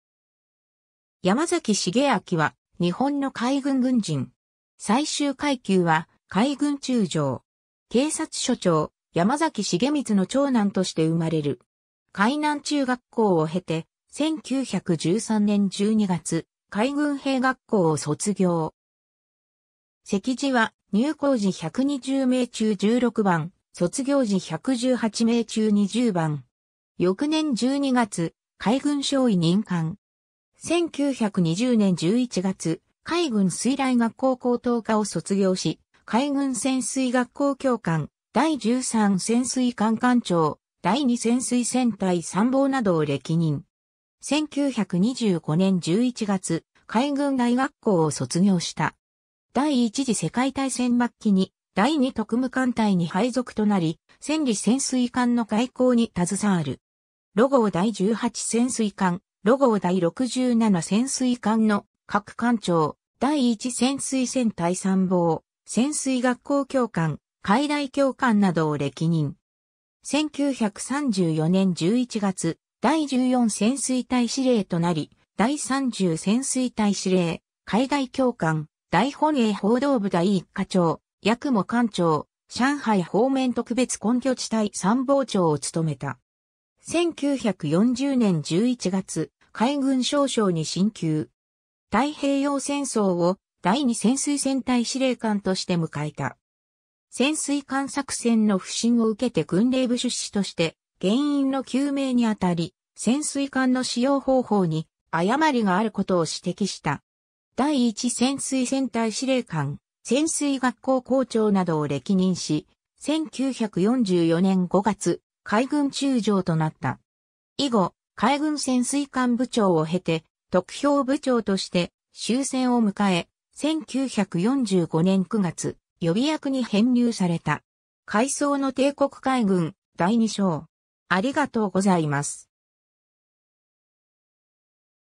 山崎重暉
山崎 重暉（やまざき しげあき、1893年1月21日 - 1980年12月28日）は、日本の海軍軍人。
 警察署長・山崎重光の長男として生れる。
海南中学校を経て、1913年12月、海軍兵学校（41期）を卒業。
席次は入校時120名中16番、卒業時118名中20番。
翌年12月、海軍少尉任官。
1920年11月、海軍水雷学校高等科を卒業し、海軍潜水学校教官、「第13潜水艦」艦長、第2潜水戦隊参謀などを歴任。
1925年11月、海軍大学校（甲種23期）を卒業した。
第一次世界大戦末期に第二特務艦隊に配属となり、戦利潜水艦の回航に携わる。
 「呂号第18潜水艦」・「呂号第67潜水艦」の各艦長、第1潜水戦隊参謀、潜水学校教官、海大教官などを歴任。
1934年11月、第14潜水隊司令となり、第30潜水隊司令、海大教官、大本営報道部第1課長、「八雲」艦長、上海方面特別根拠地隊参謀長を勤めた。
1940年11月、海軍少将に進級。
 太平洋戦争を第2潜水戦隊司令官として迎えた。
潜水艦作戦の不振を受けて軍令部出仕として原因の究明にあたり、潜水艦の使用方法に誤りがあることを指摘した。
第1潜水戦隊司令官、潜水学校校長などを歴任し、1944年5月、海軍中将となった。
以後、海軍潜水艦部長を経て、特兵部長として終戦を迎え、1945年9月、予備役に編入された。
 ^ 『回想の帝国海軍』第二章